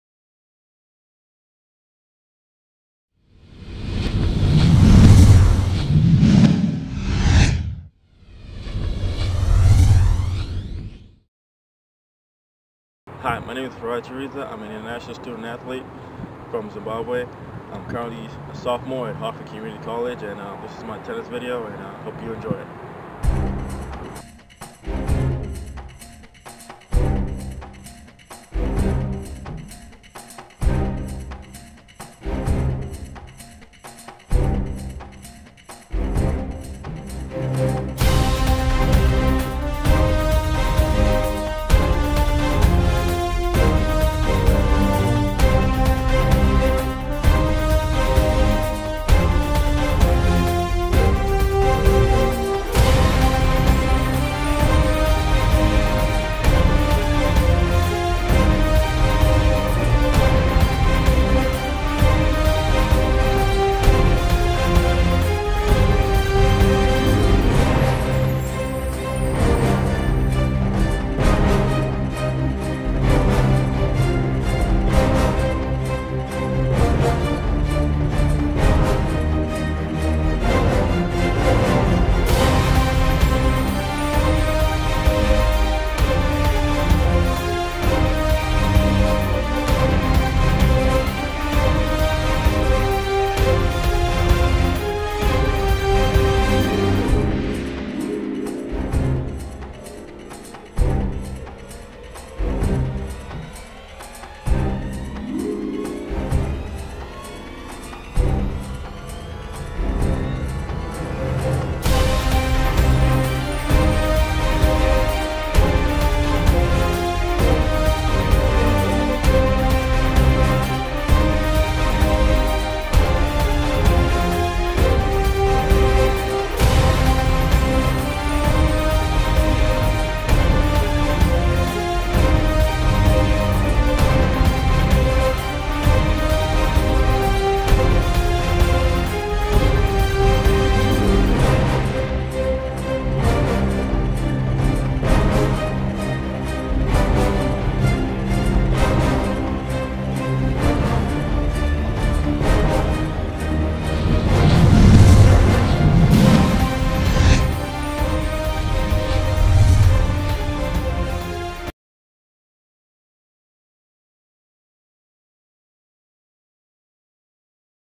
Farai Chiridza (Men's Tennis 2013-2014)
Farai Chiridza
Upcoming Junior, (2013 Grad from Harford Community College)
Intended Enrollment: Fall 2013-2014 (August)